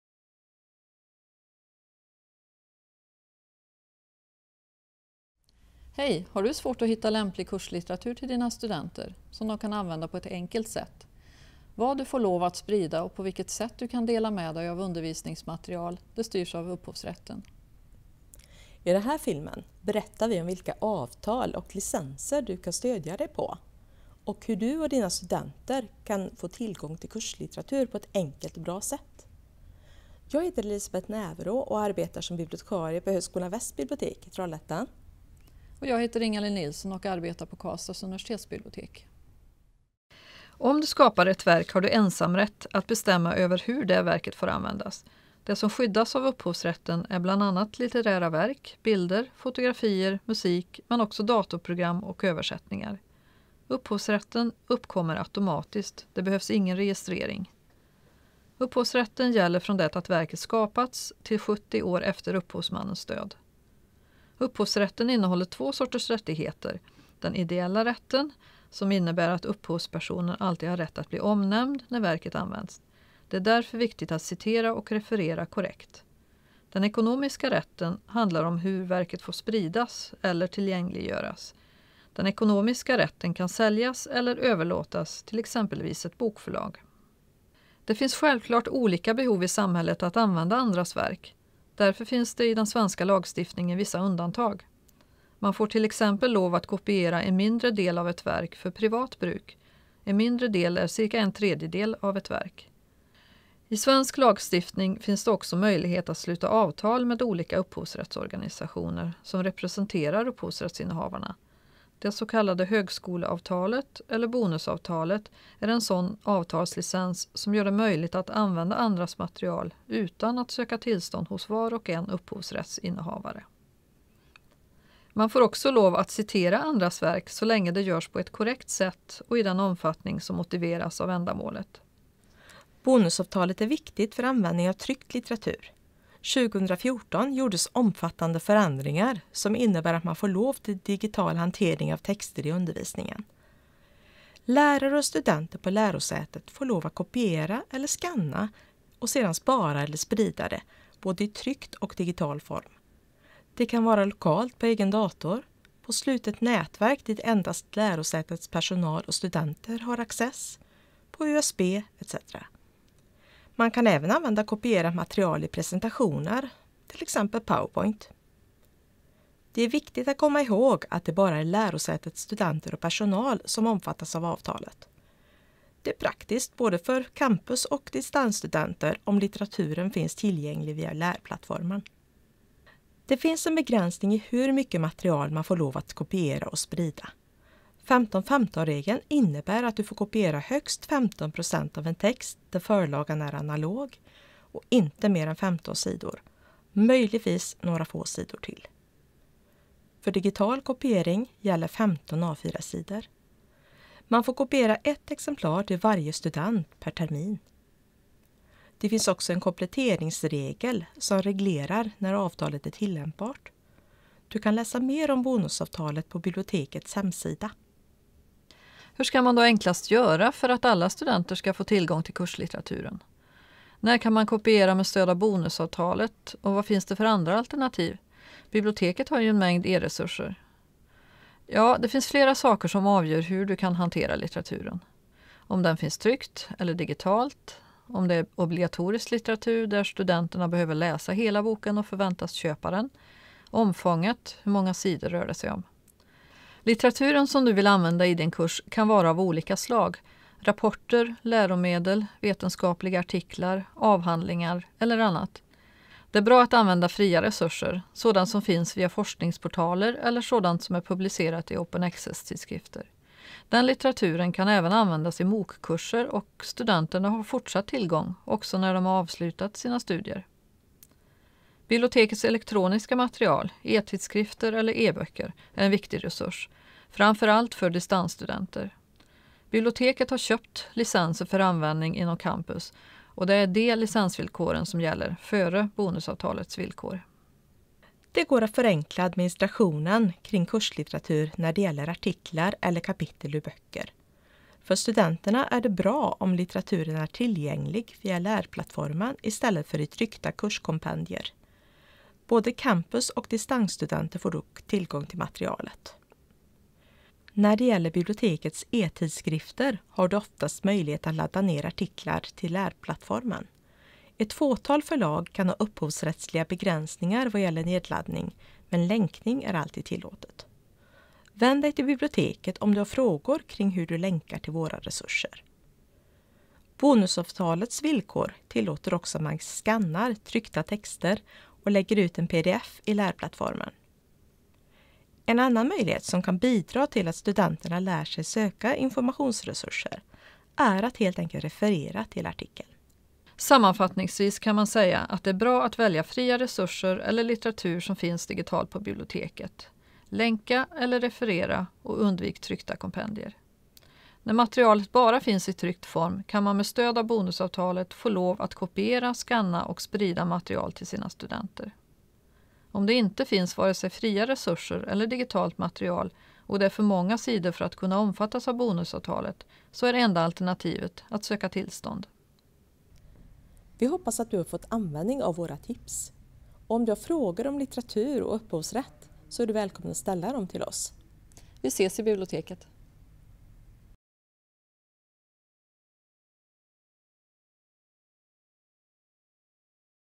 Kurslitteratur och upphovsrätt riktat till dig som är lärare
Vad ska man tänka på när man väljer kurslitteratur? Hur mycket får man kopiera och dela? Ta del av två bibliotekariers tankar och råd genom denna film